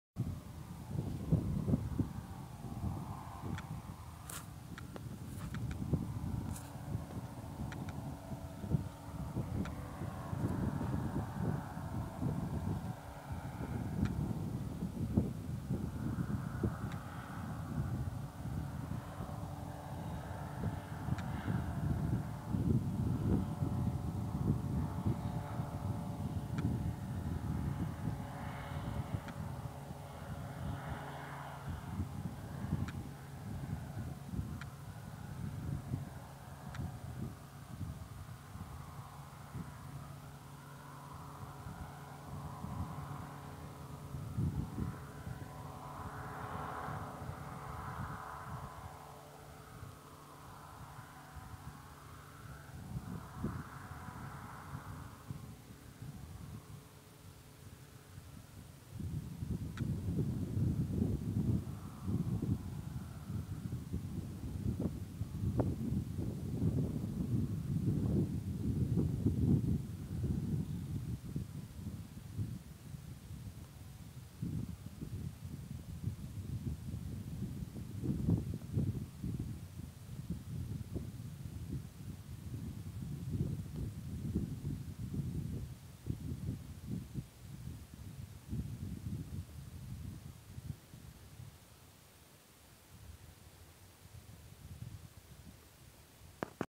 Дельтоплан
Дельтоплан над деревней